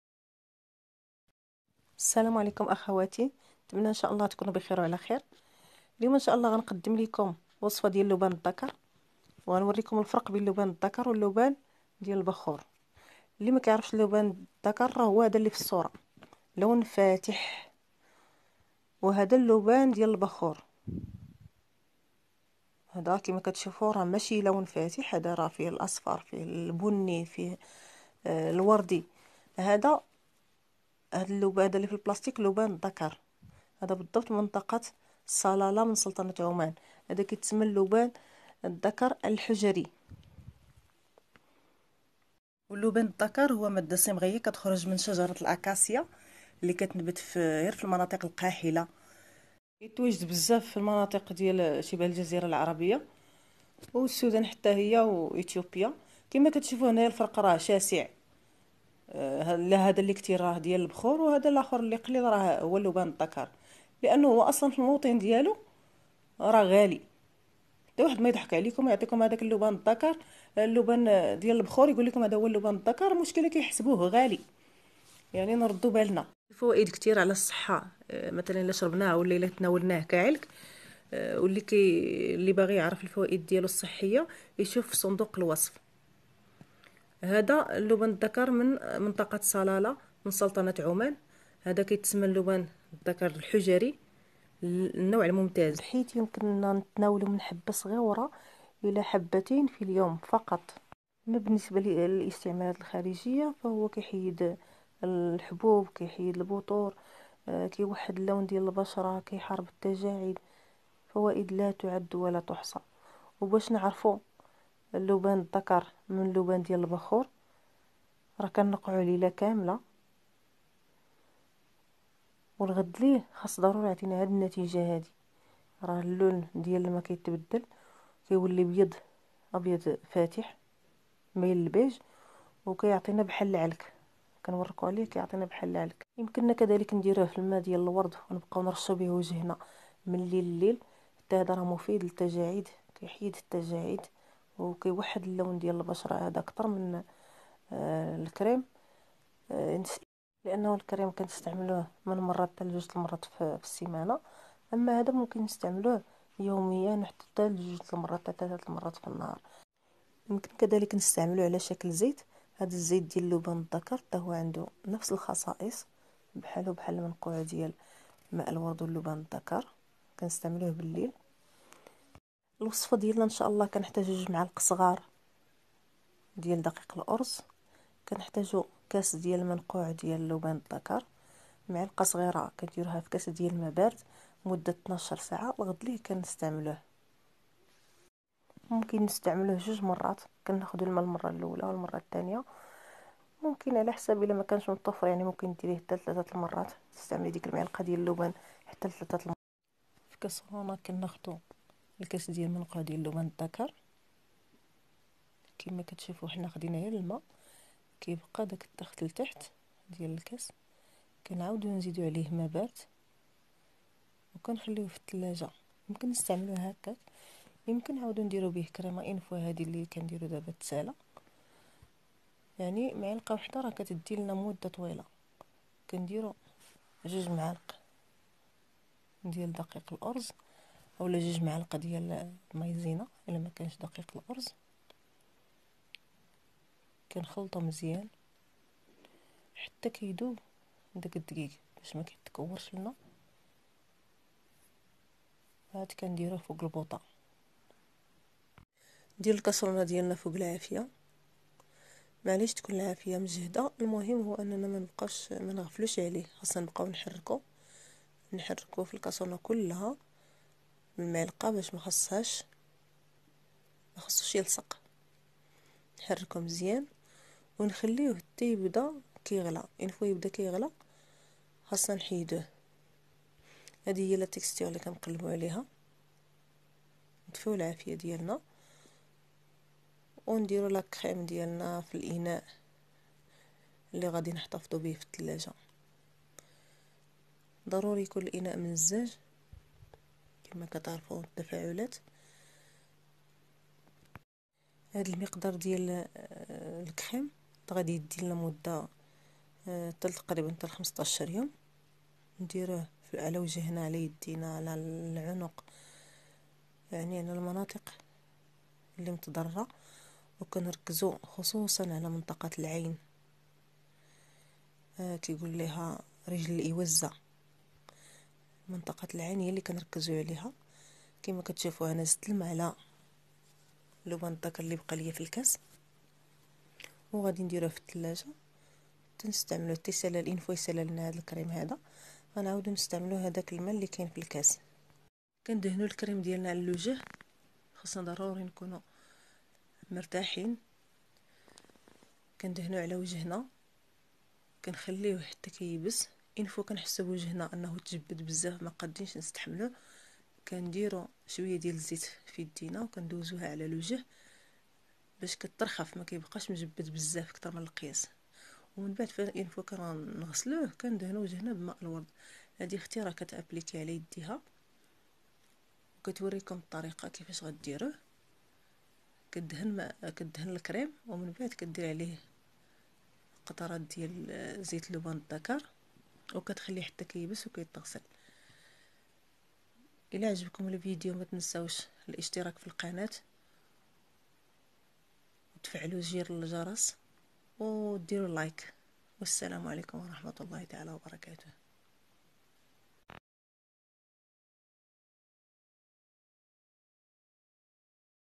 تعرفي على الفرق بين اللبان الذكر و لبان البخور و على الكريمة السحرية ضد التجاعيد......
اللبان الذكر  :
اللبان الذكر ليس بزهرة ولا بثمرة بل هو إفرازات شجرة الأكاسيا التي تتغدى عليها  الجمال  وهذه  الشجرة تنبت في المناطق القاحلة التي ليس فيها اي نباتات  وهي تأخد اكبر طاقة من الشمس إذن فقوة تلك الشجرة كلها تخرج مع هذه الإفرازات  وهنا تكمن أهميتها الطبية  .

 تعتبر دول الجزيرة العربية ودول الحبشة وإثيوبيا  و السودان من  أكبر البلدان المنتجة للبان الذكر  وعلى رأسها سلطنة عمان وحتى في بعض البلدان الأسيوية كأندونيسيا وماليزيا، غير أن اللبان الذكر  العماني من بين أجودها.

وتتجلى أهمية اللبان الذكر  في قيمته العلاجية حيت يتم تناوله على شكل علك بمقدار حبة صغيرة إلى حبتين صغيرتين فقط في اليوم ، فكله منافع ويعتبر من بين الأشياء التي :
 *  تواجه التأكسد الداخلي في الجسم .
* تخفظ الكسترول في الجسم.
* المفيدة  للجهاز الهضمي .
*تحفظ بعض الإفرازات الخبيثة في الجسم  .
* تكبح السرطان.
* تريح الكلى.
* مفيد لأمراض القلب والشرايين.
* جيد لأمراض الجهاز العصبي :
النسيان . الاردنيون . آلزاهيمر .
اما الاستعمالات الخارجية فإستعماله كويت أو كريم يساعد على علاج مشاكل البشرة من توحيد اللون ومحاربة التجاعيد والبثور .إلخ